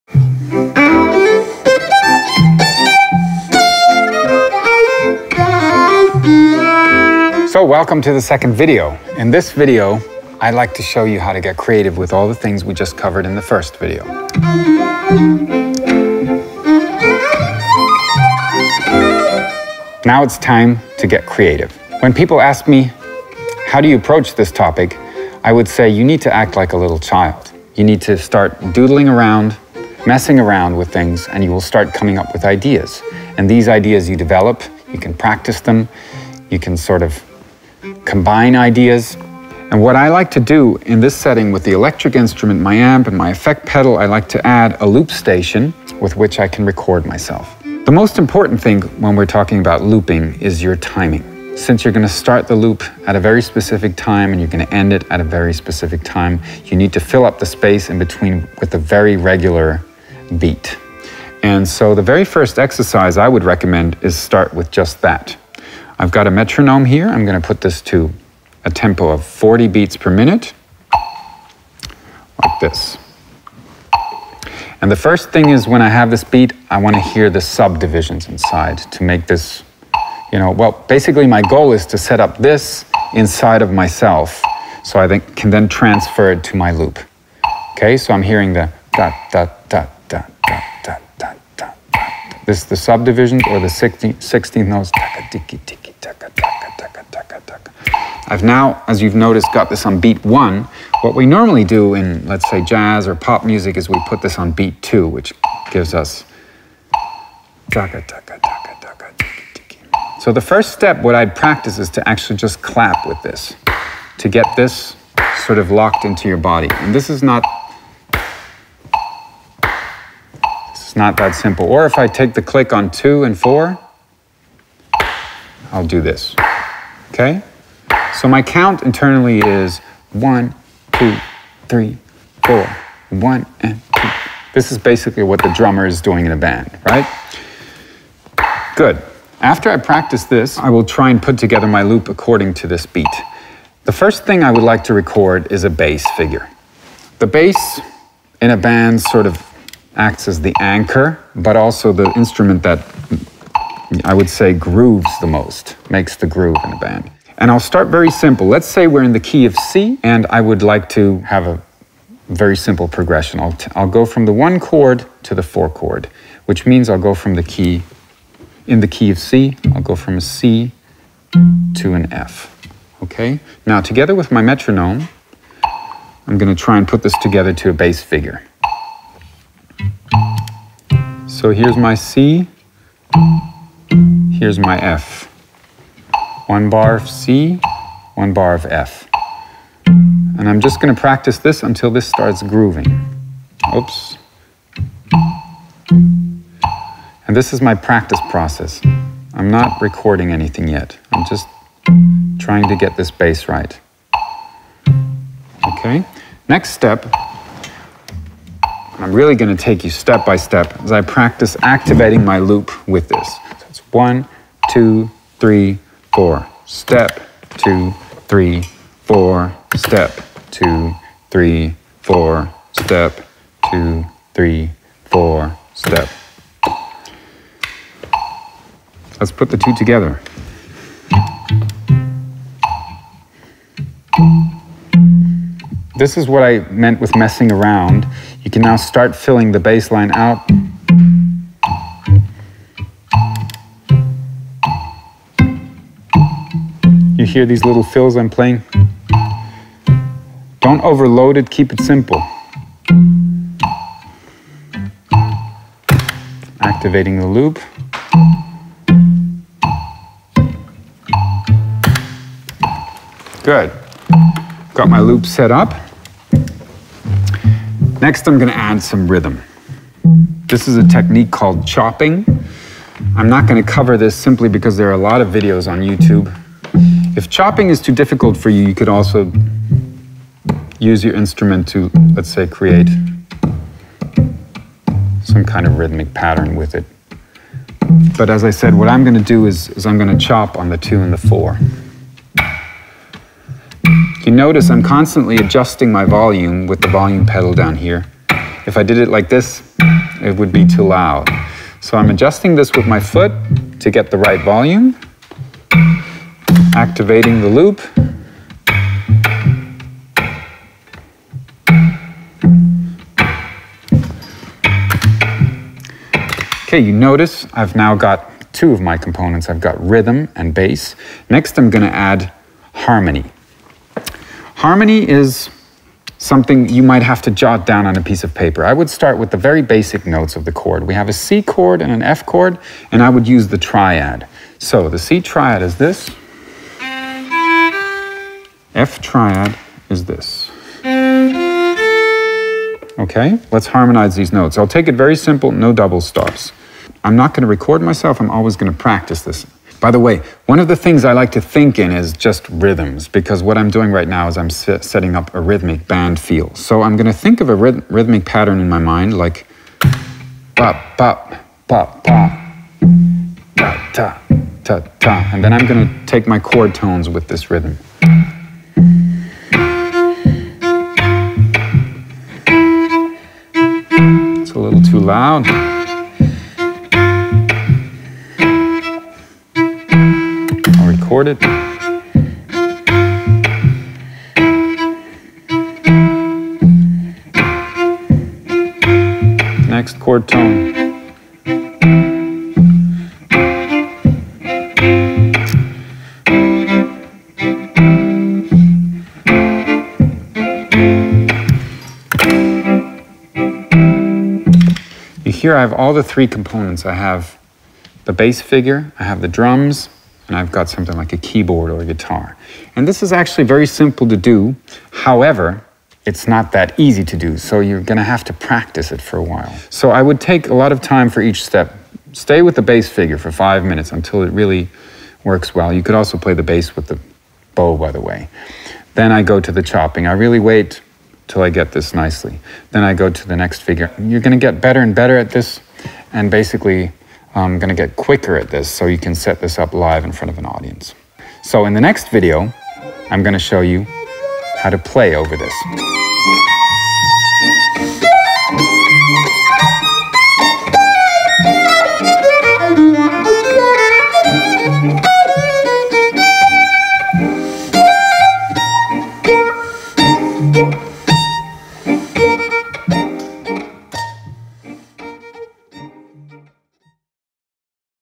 Silent String Masterclass with Max Grosch - Episode 2 | Yamaha Music
Did you always wanted to try out an electric violin but didn't know where to start?
In this 5-episodes masterclass Yamaha Artist Max Grosch, will unveil all the basics you'll need to expand your musical horizon to electric and silent strings.